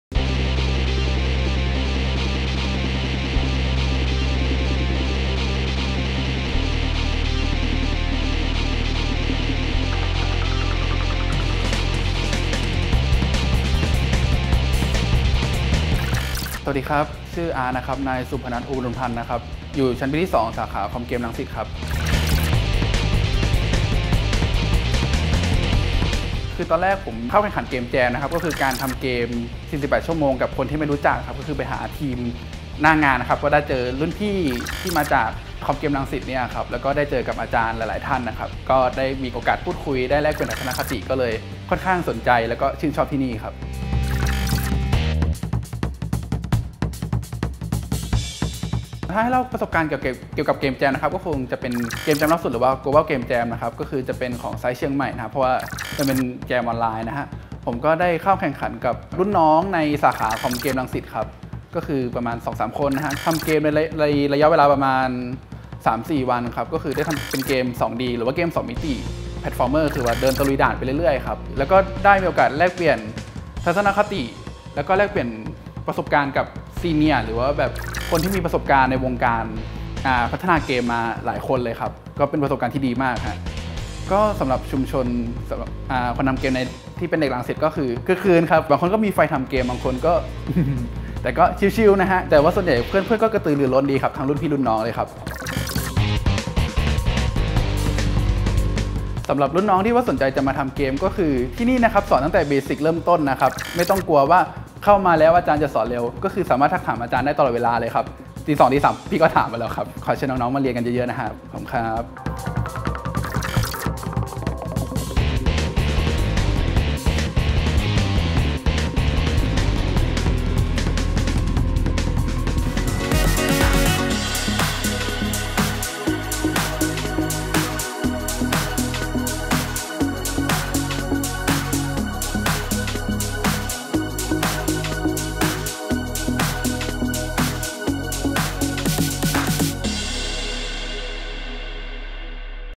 RSU Computer Game and Esport : ศุภณัฐ อุบลพันธ์ (อาร์ต)
ศุภณัฐ อุบลพันธ์(อาร์ต) นักศึกษาวิทยาลัยนวัตกรรมดิจิทัลเทคโนโลยี
สาขาวิชาคอมพิวเตอร์เกมส์และอีสปอร์ต มหาวิทยาลัยรังสิต [ Computer Game and Esport ]

สิ่งที่เราถนัด : เขียนโปรแกรมและออกแบบเกมส์